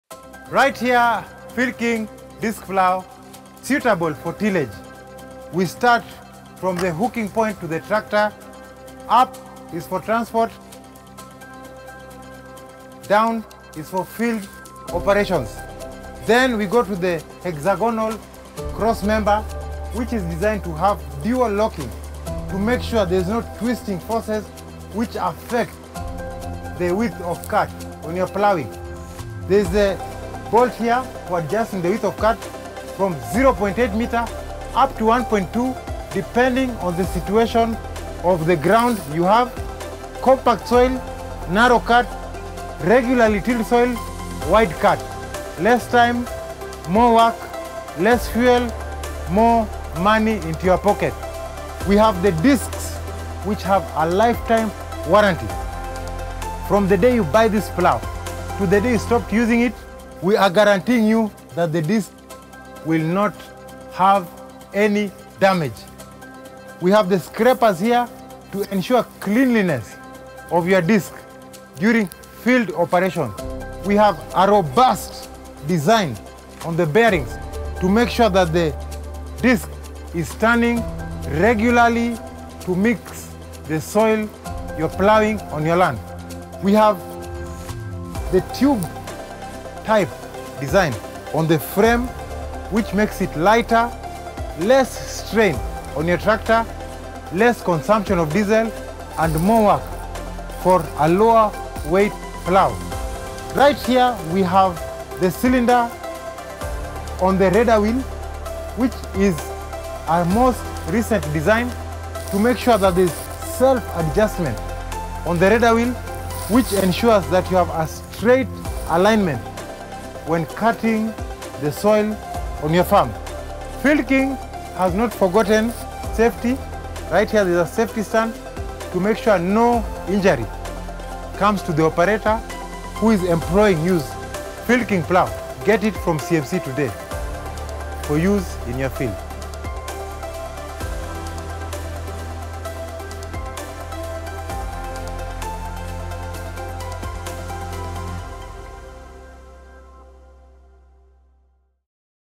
FieldKing Implements - The Disc Plough (Jembe)
FieldKing offers farm implements that are built tough. A great choice for Kenyan farmers!

Here's some quick facts on the FieldKing 2 Disc Plough:

✔️It is directly mounted to the tractors.
✔️It can be use easily at rocky and rooted areas.
✔️It is used to open the new fields and to process the stony areas.
✔️It is specially useful in hard and dry trashy land conditions and in soils where scouring is a major problem.
✔️The disc plough is designed to work in medium & light soil for basic functions such as soil breaking, soil raising, soil turning and soil mixing.